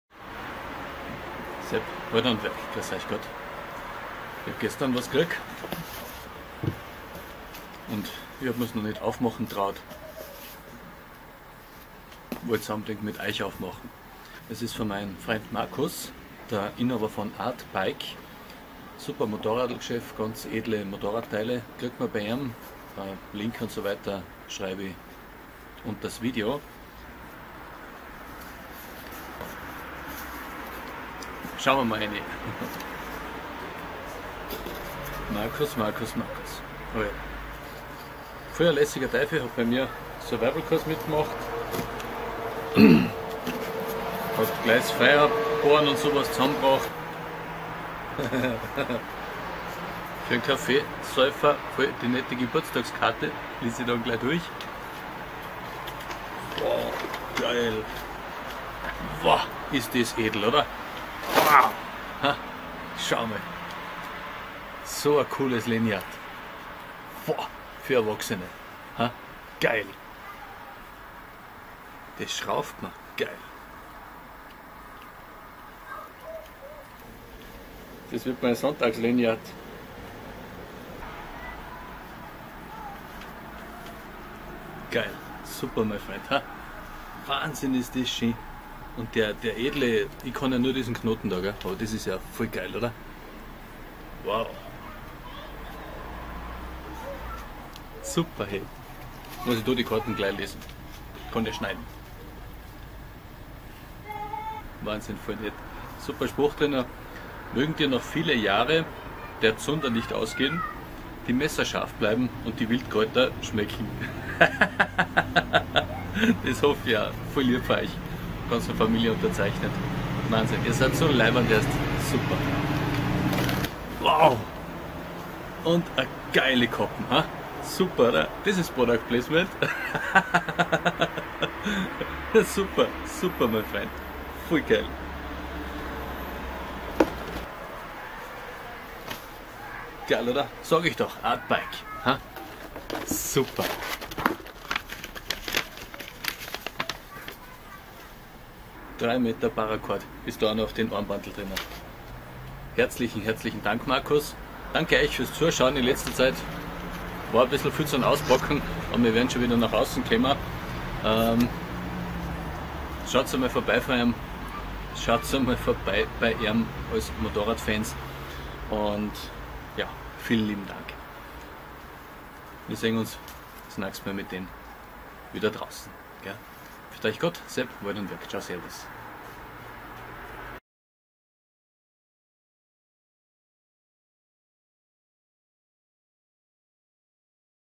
Bracelet und Kappe von art biketech com
1000 Dank mein Freund, ein super schönes selbstgemachtes Paracord Survival Armband und eine fesche Kappe!
Sepp mit Familie.